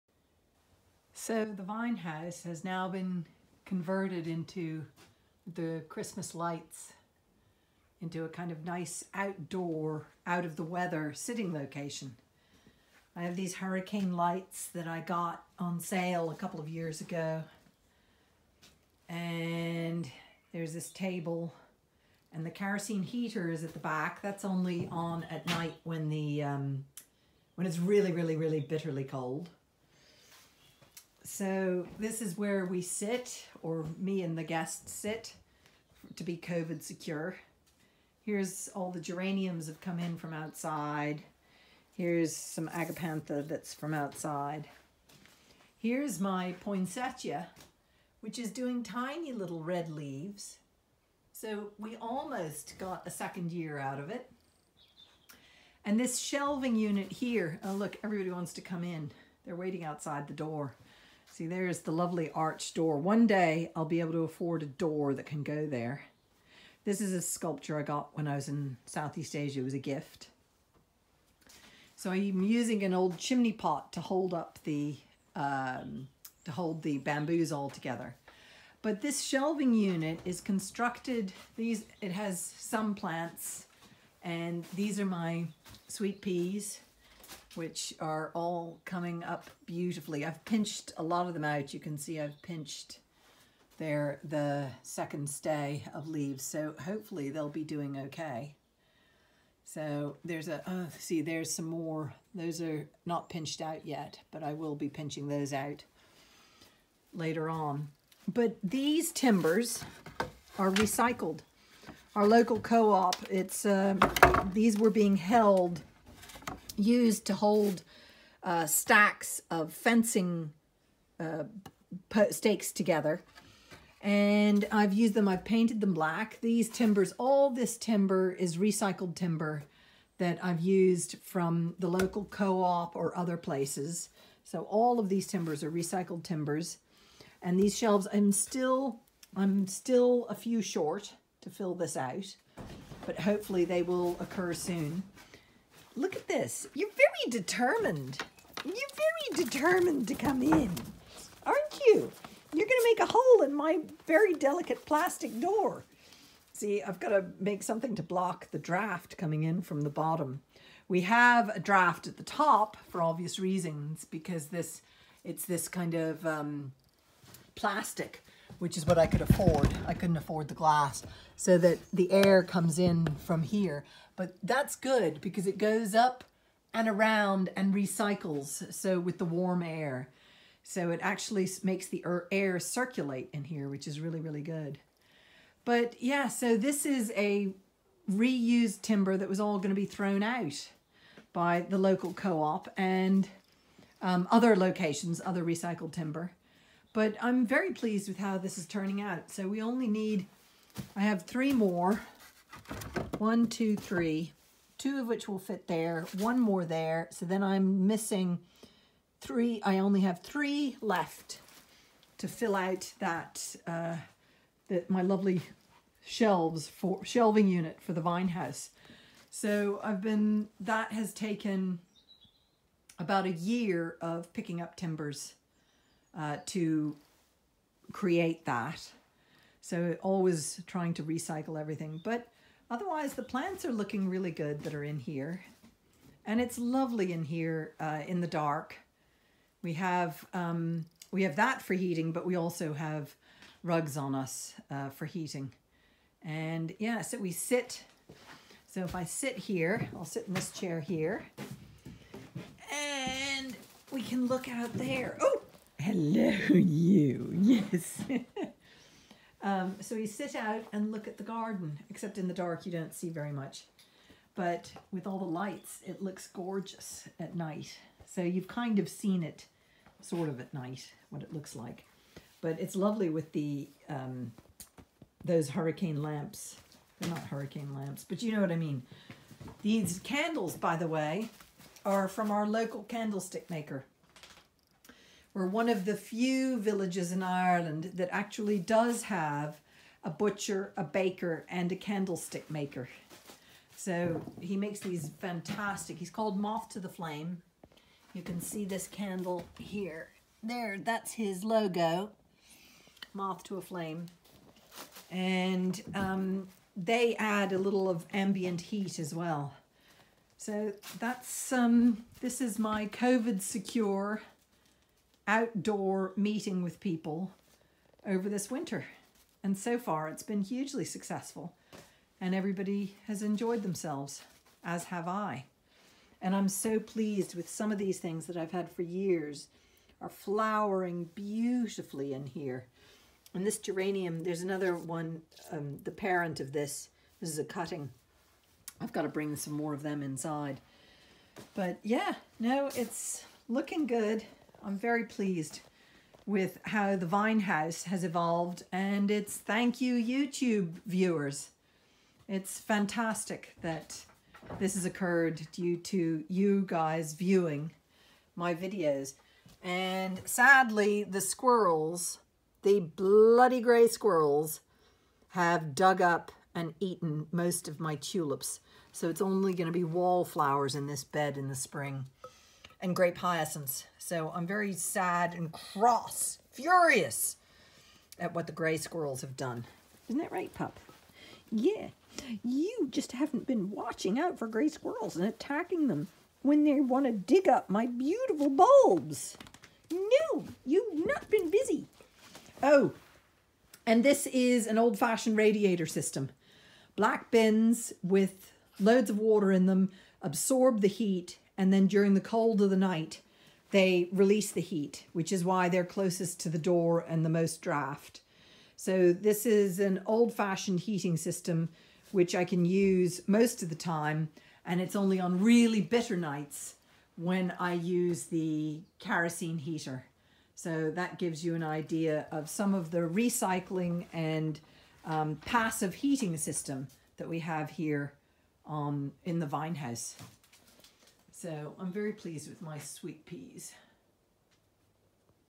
Vine-house ready for seedlings & bad weather covid safety for visitors
I love my up cycled shelving unit. Old fashioned natural heating system of Solar barrel heating is what’s mostly heating the vine house. I only use the kerosene heater on really cold windy nights that might suck the ambient black water barrel heating system’s heat out of the vine-house faster.